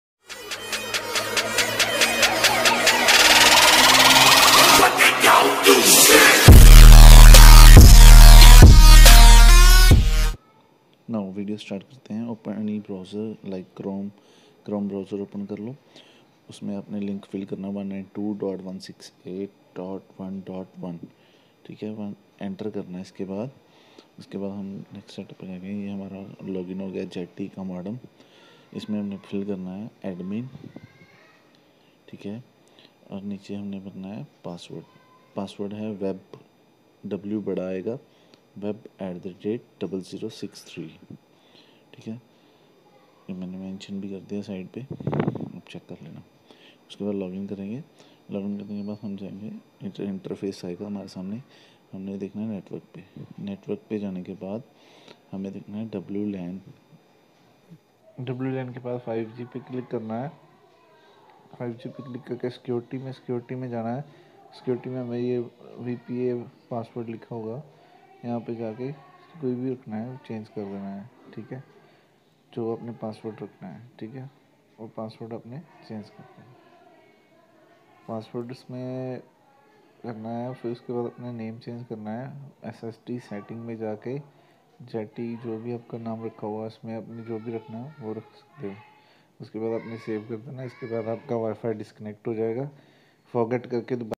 How to Change Wi-Fi Password ONLY for ZTE 5G Model | Modem (NetPlus Broadband)
Net Plus Customer Care no. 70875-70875
Warning: please Reset Your Modem if it's ZTE Model Only , Don't Try with other Modem and first call to customer care , Friends don't Blame me,if your password didn't change.i just try to tell how I changed my WiFi password In this video